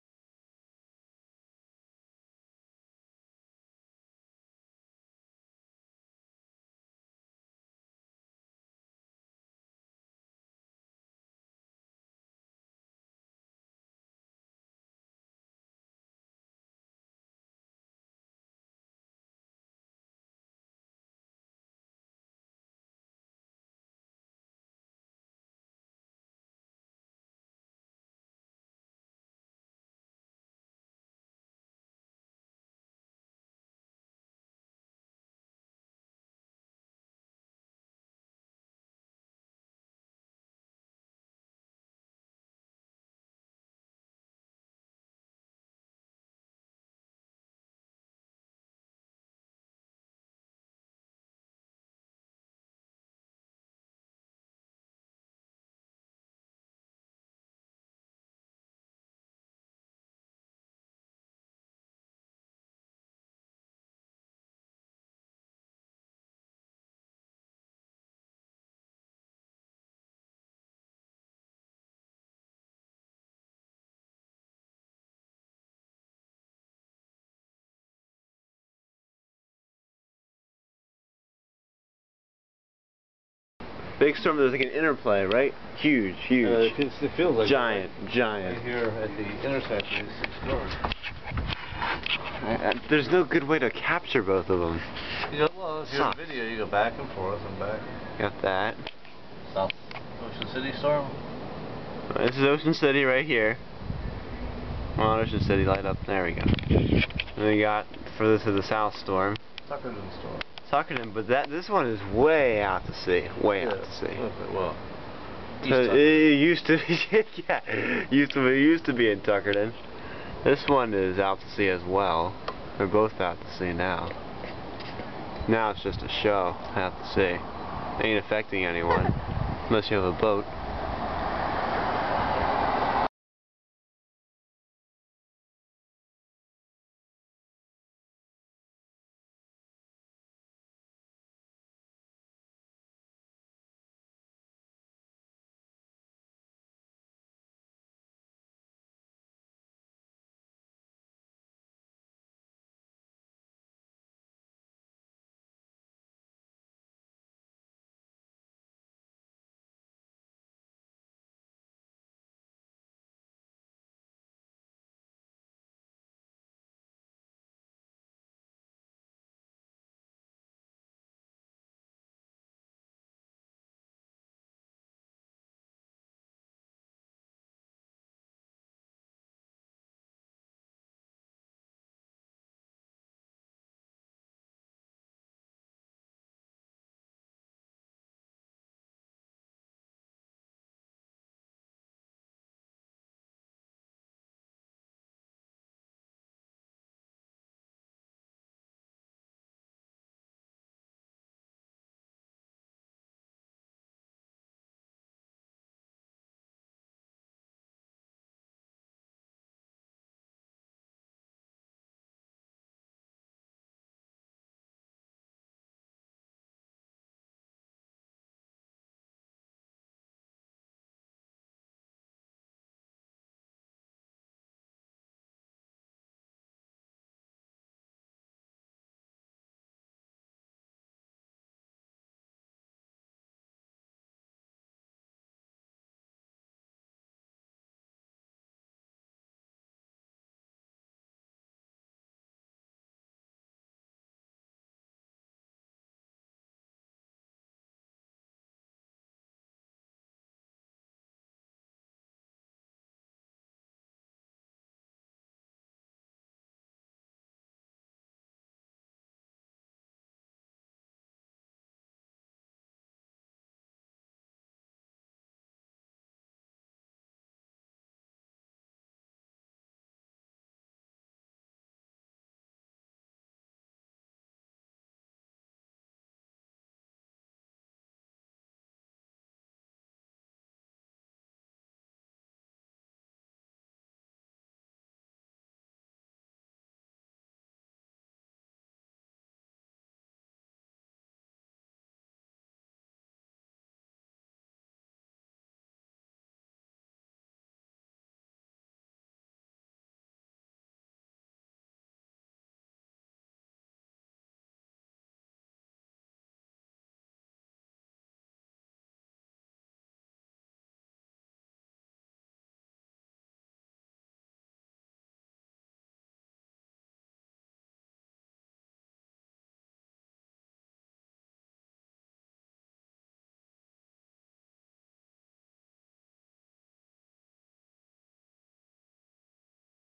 Crazy Lightning Show in Brigantine (Part 2)
More beautiful lightning from that same night...
Note: At about the 1:25 mark, I do give some context to where the storms are and what the situation is.